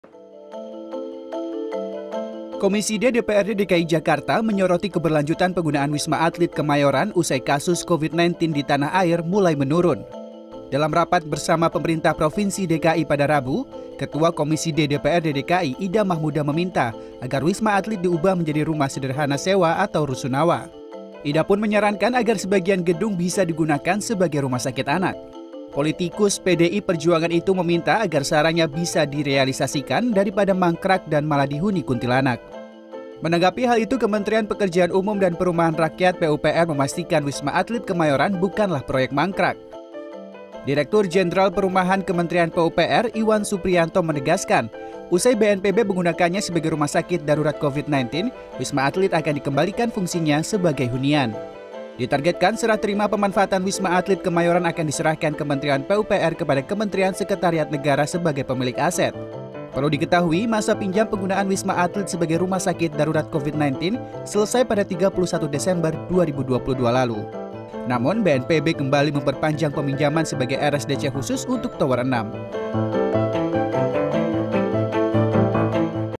DPRD DKI: Wisma Atlet Lama Kosong, Banyak Kuntilanak
Ketua komisi D DPRD DKI Jakarta, Ida Mahmunah, meminta kejelasan alih fungsi penggunaan wisma atlet Kemayoran, usai dipakai sebagai rumah sakit darurat Covid-19. Ida meminta wisma atlet dijadikan sebagai rusunawa, dibandingkan mangkrak dan malah dihuni banyak kuntilanak.